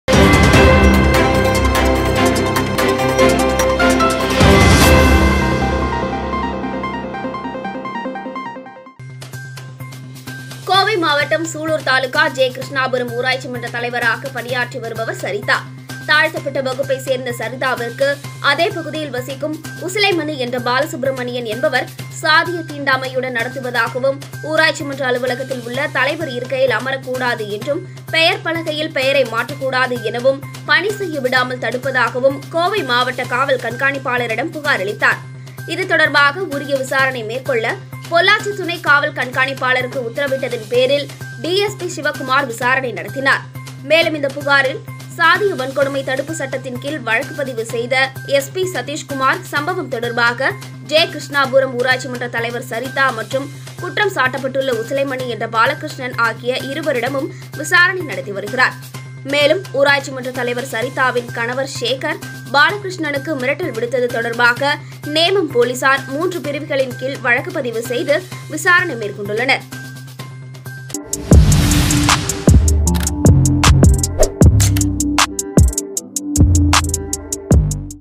தாழ்த்தப்பட்ட வகுப்பைச் சேர்ந்த ஊராட்சி மன்றத்தலைவவருக்கு சாதிய தீண்டாமை புகார்
கோவை மாவட்டம் சூலூர் தாலுகா ஜெ.கிருஷ்ணாபுரம் ஊராட்சி மன்ற தலைவராக பணியாற்றி வருபவர் சரிதா. தாழ்த்தப்பட்ட வகுப்பைச் சேர்ந்த சரிதாவிற்கு அதே பகுதியில் வசிக்கும் உசிலைமணி என்ற பாலசுப்ரமணியன் என்பவர் சாதிய தீண்டாமையுடன் நடத்துவதாகவும்,ஊராட்சி மன்ற அலுவலகத்தில் உள்ள தலைவர் இருக்கையில் அமரக்கூடாது என்றும்,பெயர் பலகையில் பெயரை மாற்றக்கூடாது எனவும்,பணி செய்ய விடாமல் தடுப்பதாகவும் கோவை மாவட்ட காவல் கண்காணிப்பாளரிடம் புகார் அளித்தார்.இதுதொடர்பாக உரிய விசாரணை மேற்கொள்ள பொள்ளாச்சி துணை காவல் கண்காணிப்பாளருக்கு உத்தரவிட்டதன் பேரில் டி.எஸ் .பி சிவகுமார் குமார் விசாரணை நடத்தினர். மேலும் இந்த புகாரின் பேரில் பாலகிருஷ்ணன் மீது சாதிய வன்கொடுமை தடுப்புச் சட்டத்தின் கீழ் வழக்கு )பதிவு செய்தடிஎஸ்பி சதீஷ்குமார் சம்பவம் தொடர்பாக ஜெ. கிருஷ்ணா புரம் ஊராட்சி மன்றத்தலைவர் சரிதா மற்றும் குற்றம் சாட்டப்பட்டுள்ள உசிலைமணி என்ற பாலகிருஷ்ணன் ஆகிய இருவரிடமும் விசாரணை நடத்தி வருகிறார். மேலும் ஊராட்சி மன்ற தலைவர் சரிதாவின் கணவர் சேகர் பாலகிருஷ்ணனுக்கு மிரட்டல் விடுத்தது தொடர்பாக நேமம் போலீசார் 3 பிரிவுகளின் கீழ் வழக்கு பதிவு செய்து விசாரணை மேற்கொண்டுள்ளனர்